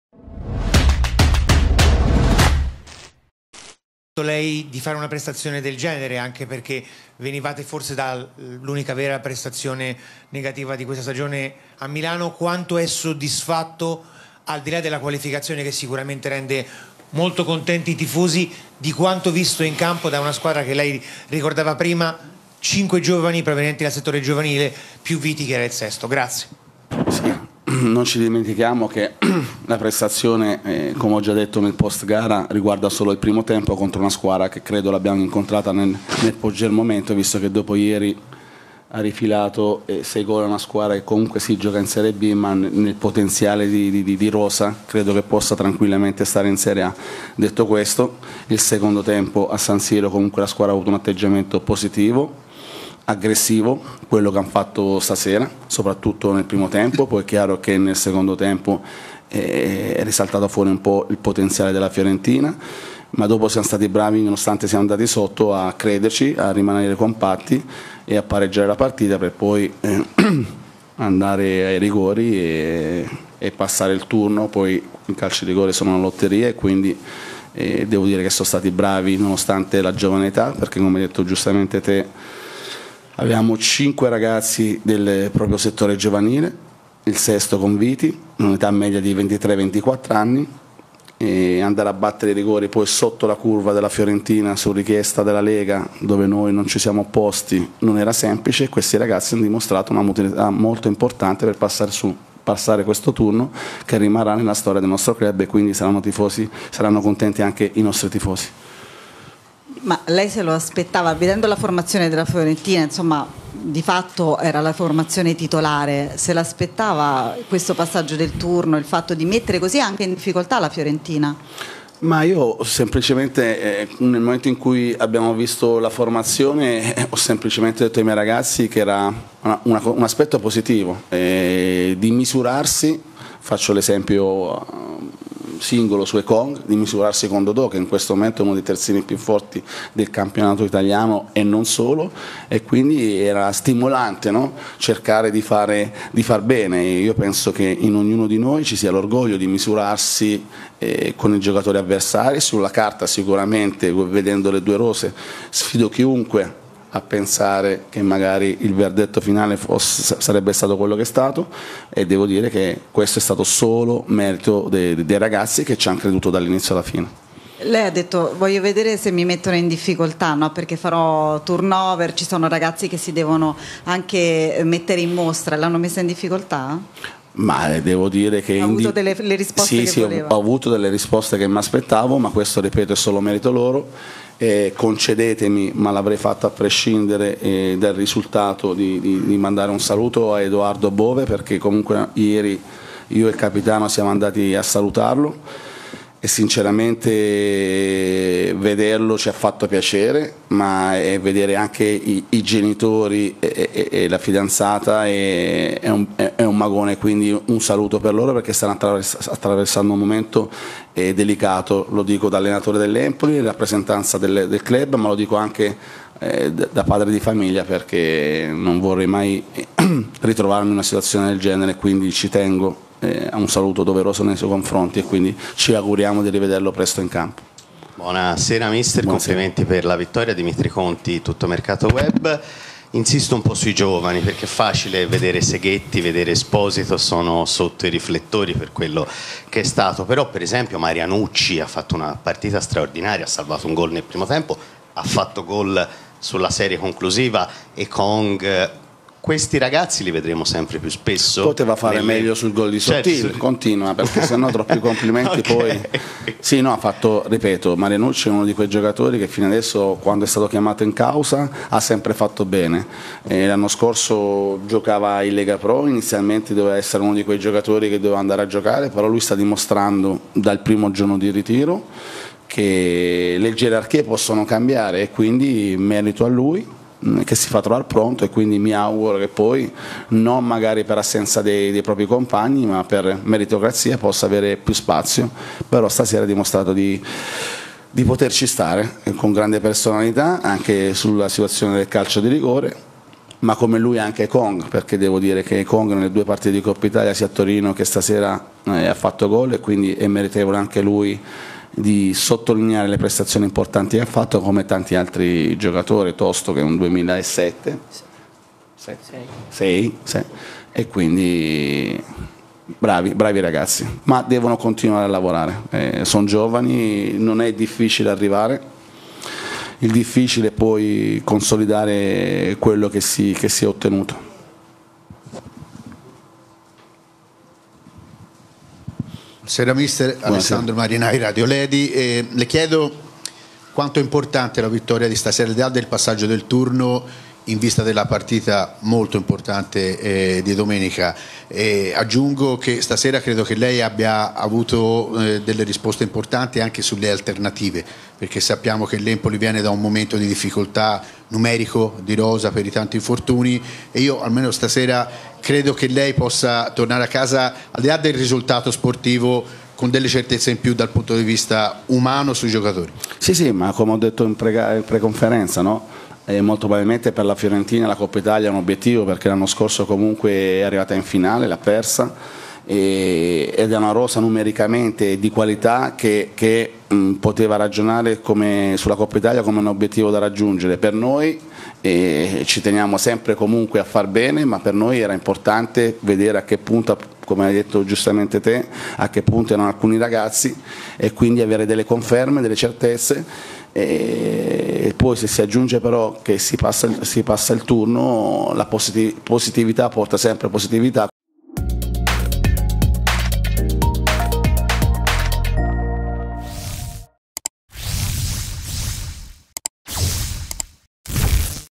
Empoli Roberto D'Aversa: "Gara che rimarrà nella storia" post Fiorentina-Empoli 3-4 Coppa Italia
Speravamo di venire e fare questa prestazione. I ragazzi hanno compiuto qualcosa di straordinario, questa partita rimarrà nella storia del club e nella memoria dei tifosi. In squadra c'erano cinque ragazzi prodotti dal nostro vivaio, sei con Viti. Hanno fatto tutti una grande partita contro i titolari della Fiorentina. Ci siamo misurati con i titolari e abbiamo ben figurato”.